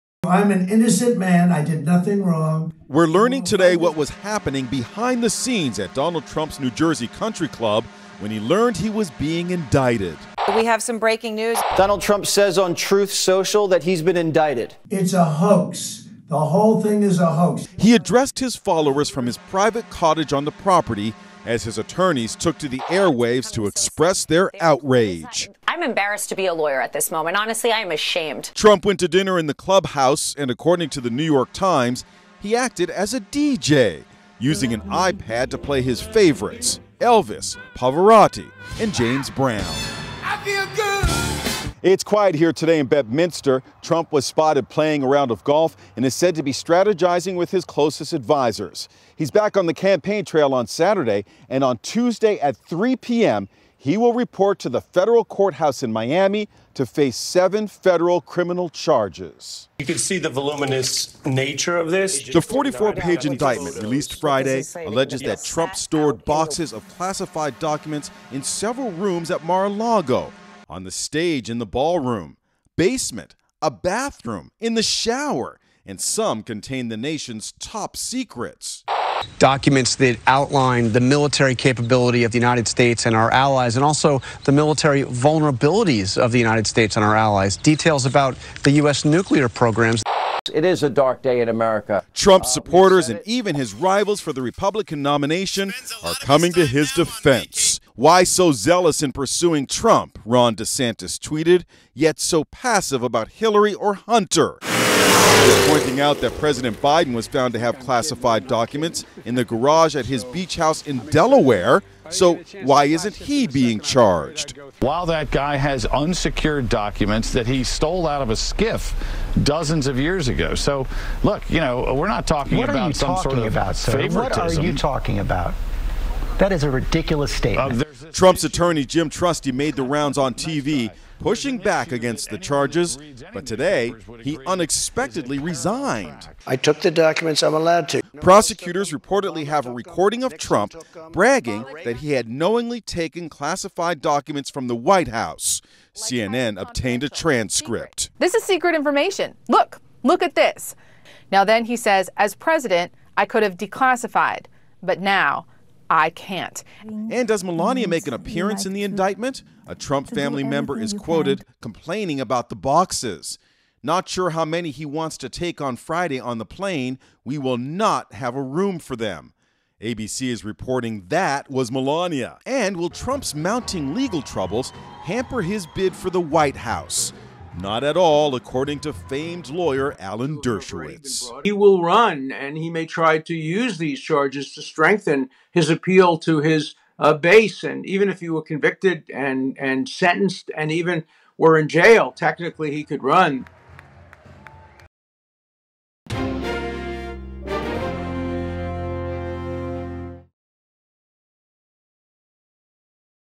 Donald Trump Indicted for Retaining Classified Documents
Former President Donald Trump declares he's an innocent man after being indicted on seven counts of mishandling classified documents. The 44-page indictment contains photos of the boxes Trump and his staff took from the White House stacked in various rooms in his Florida Mar-a-Lago estate. Prosecutors reportedly obtained a recording of Trump bragging that he had knowingly taken classified documents from the White House. Inside Edition's Les Trent has more.